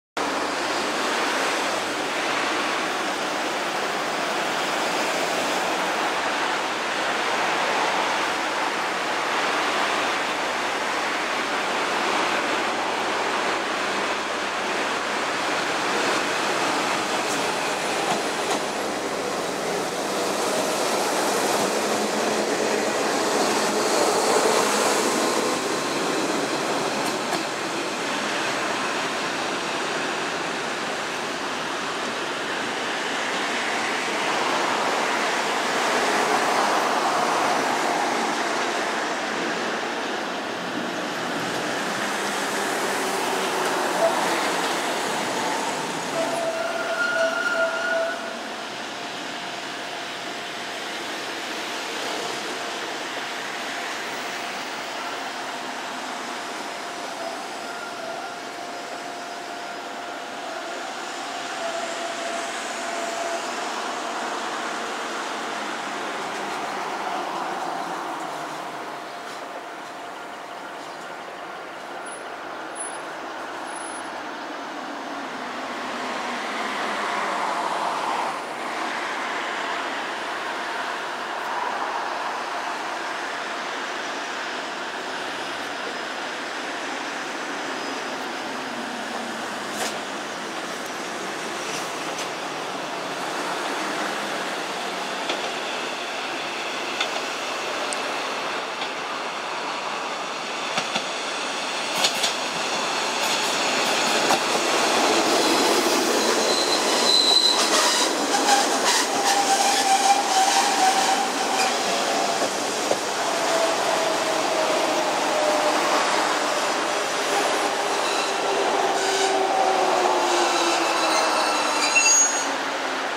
関東鉄道 キハ0形 001+002&キハ310形 316+315 ゆめみ野駅
2019年撮影
キハ0形 普通　水海道行き　キハ310形 普通　取手行き
関鉄・常総線　ゆめみ野駅ホームから